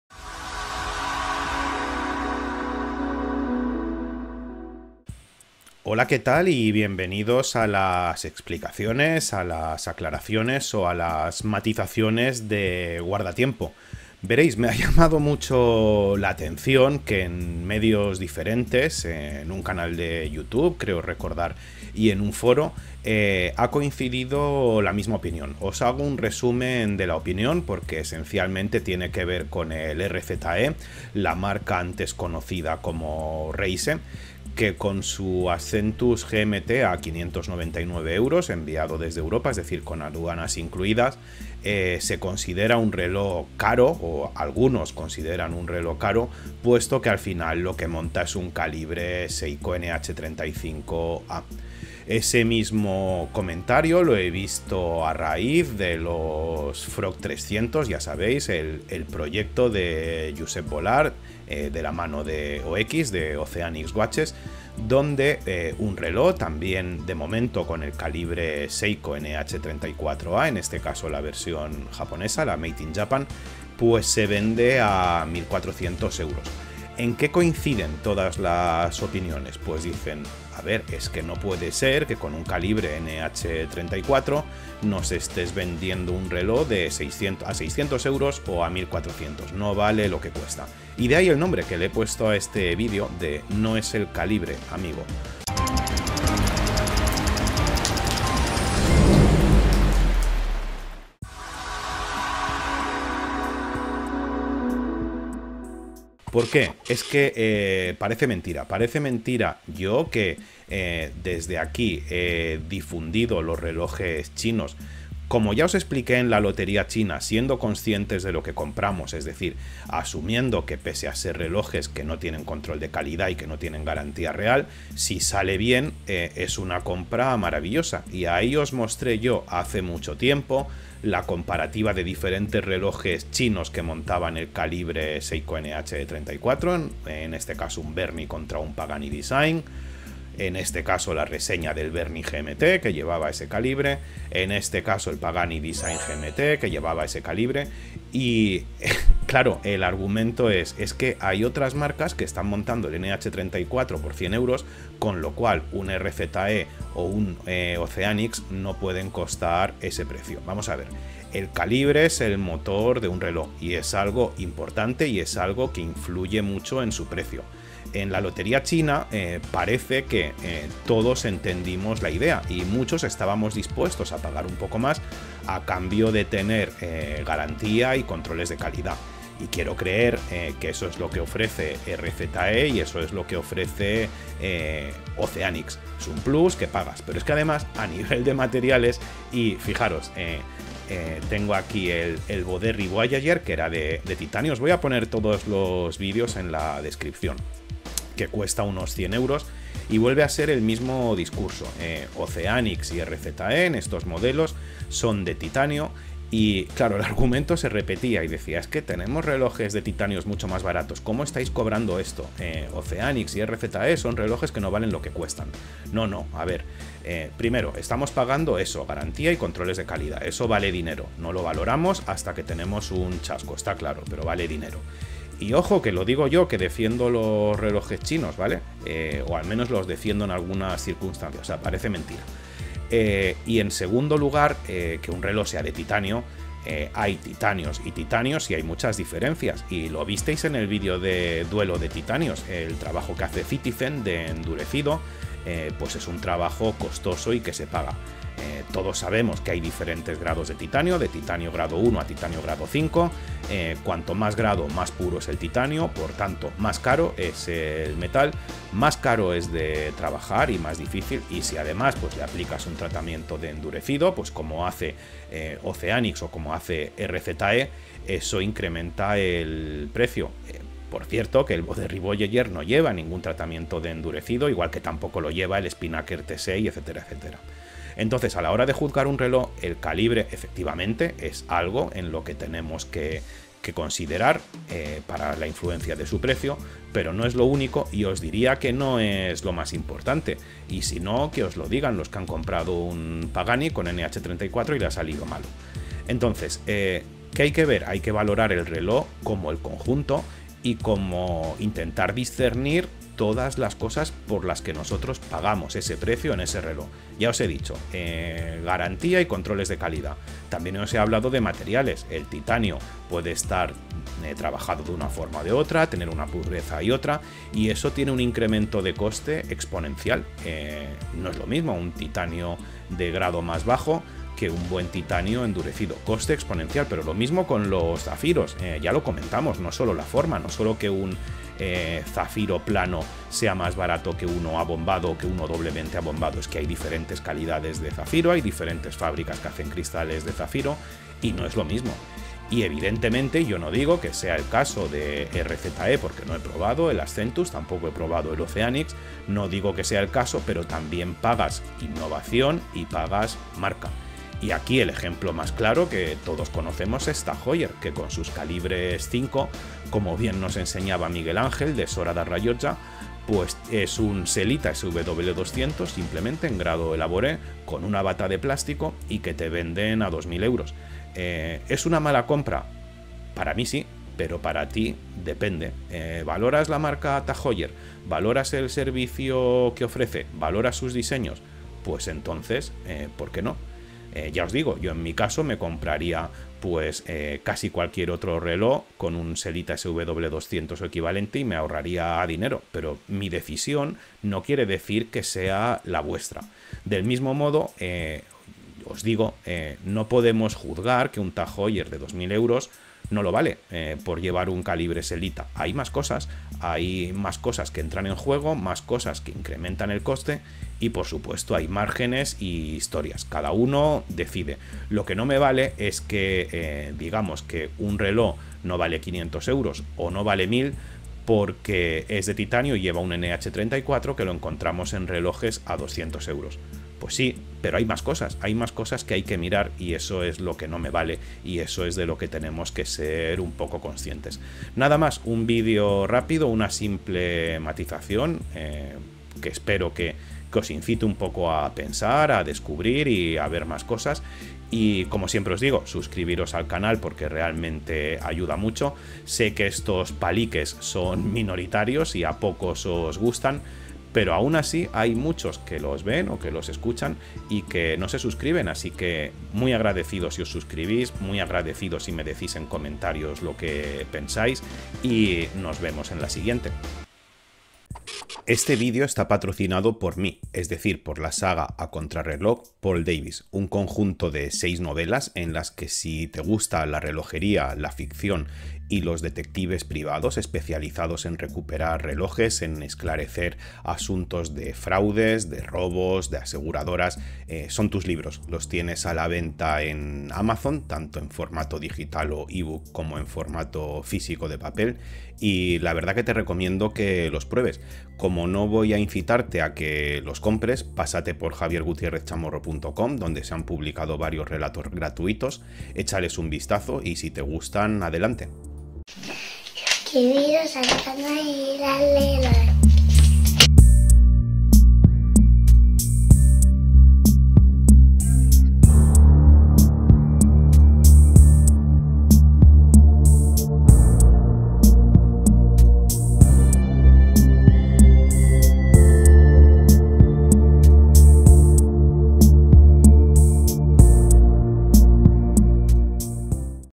No es el calibre, amigo
A cuento del RZE/Reise Ascentus GMT (@rzewatches9743 ) que se vende a 599€ y del OX/Oceanix FROG300 a 1400€ (@watchwonders) el veredicto de algunos es que son relojes que no lo valen, puesto que hay relojes con ese mismo calibre NH34A se venden a 100€.

Así que os diría que "No es el calibre, amigos", o al menos no es solamente eso. ¿Qué incrementa precio en un reloj? ¿Es el movimiento o la máquina lo más importante? ¿Es lo único?